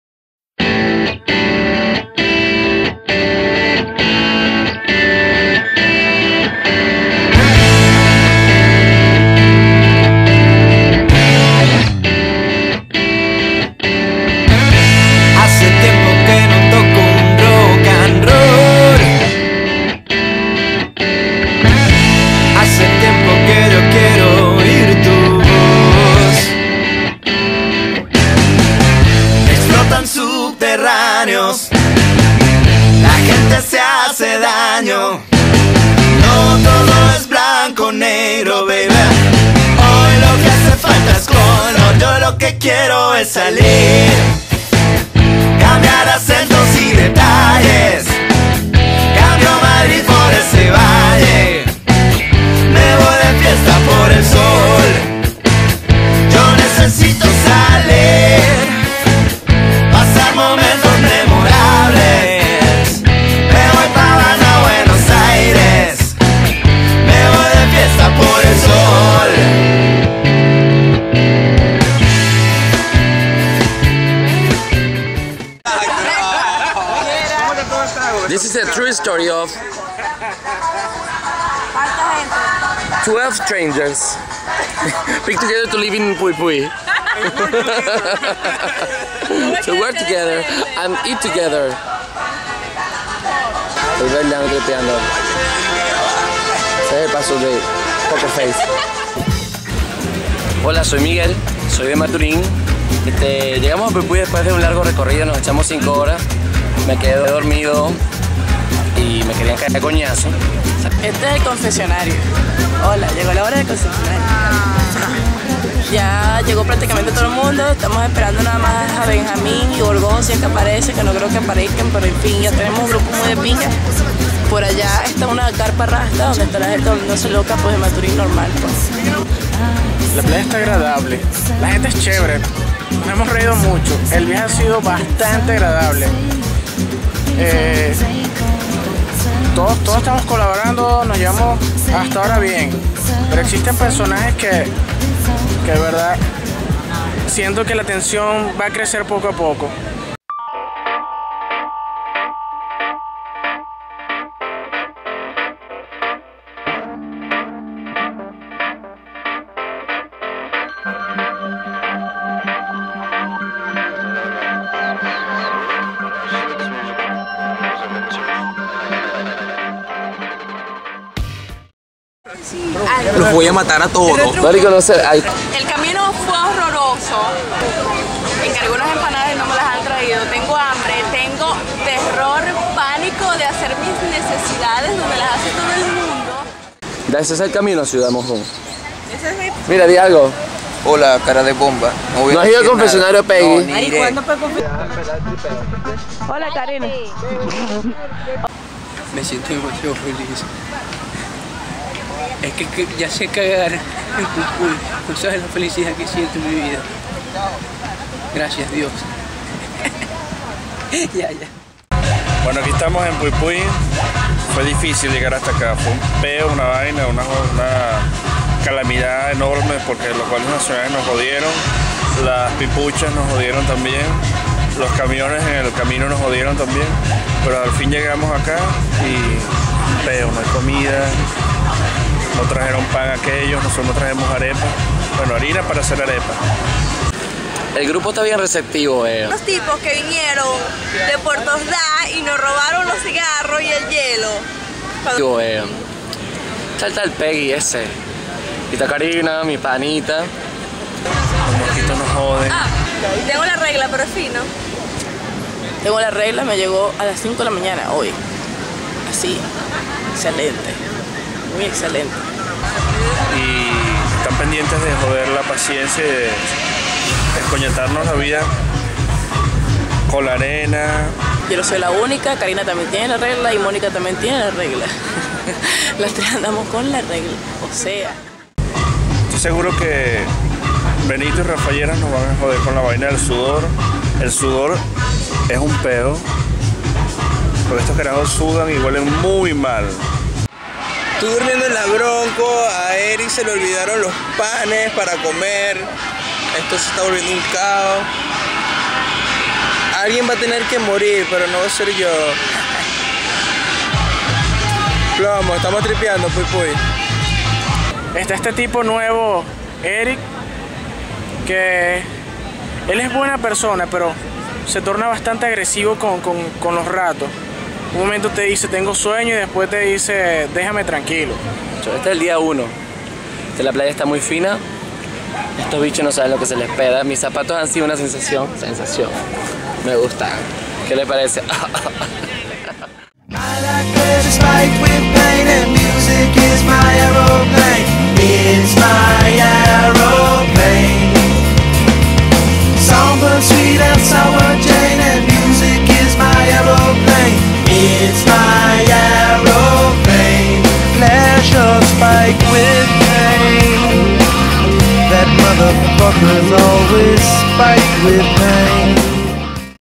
Viaje a la Playa 2010: Cronicas de Pui Puy - Episodio 1
Reality Maturin:  Viaje a la Playa 2010: Cronicas de Pui Puy - Episodio 1
Viaje de Semana Santa 2010 de unos panas de Maturin Venezuela.

Ziguaraya Producciones.